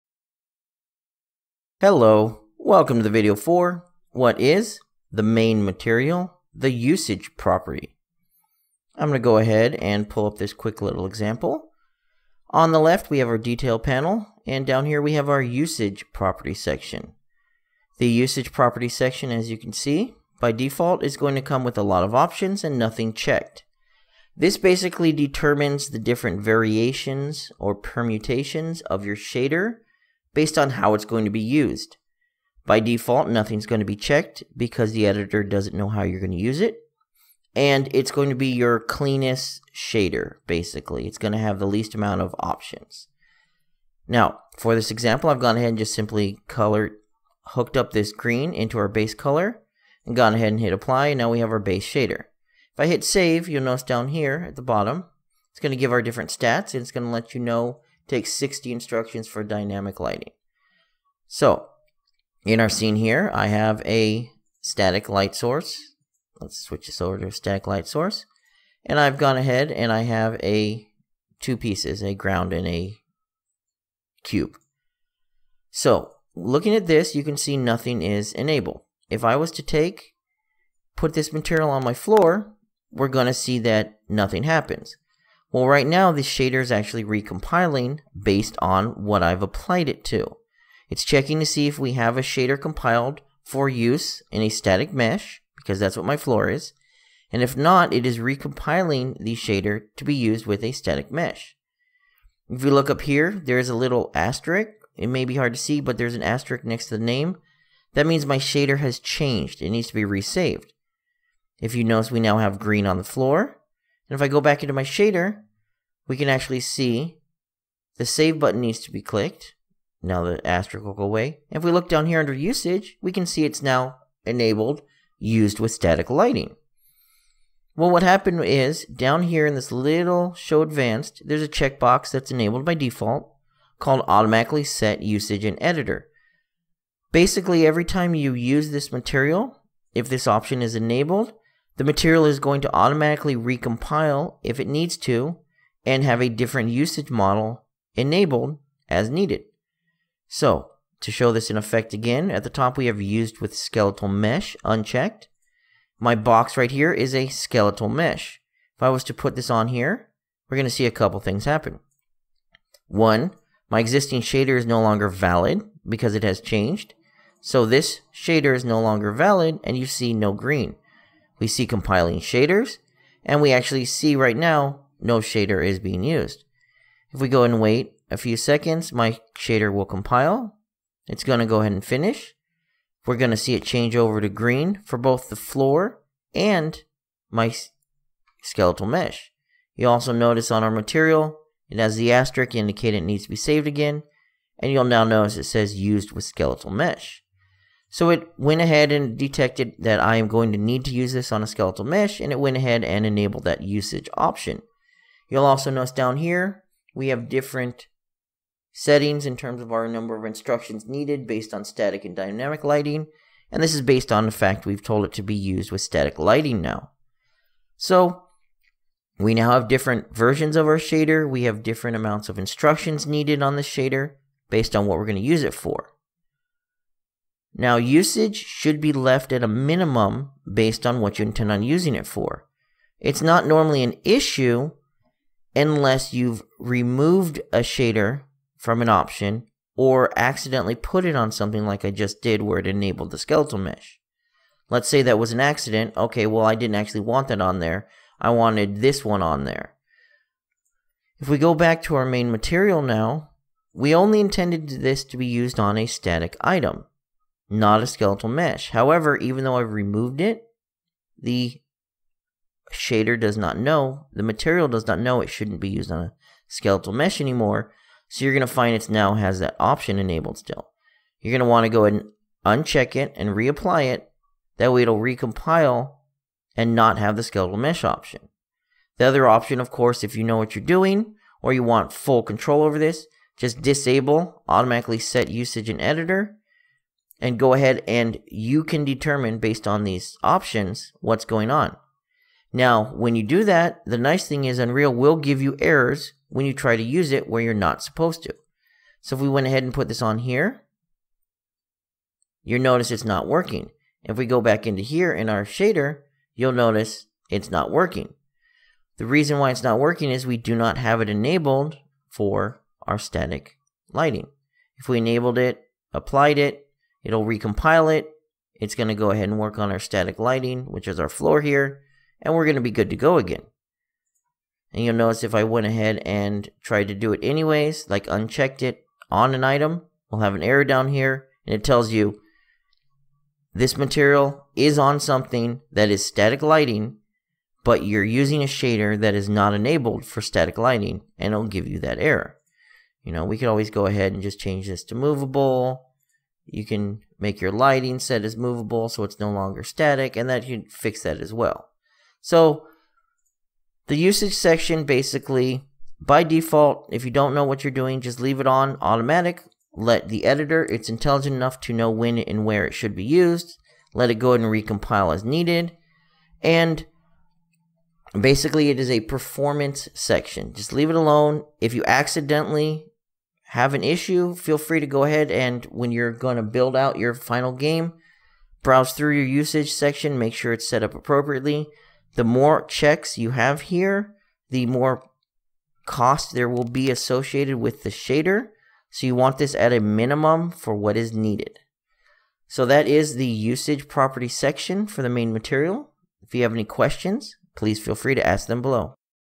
WTF Is? Main Material - Usage Property in Unreal Engine 4
What is the Main Material - Usage Property in Unreal Engine 4.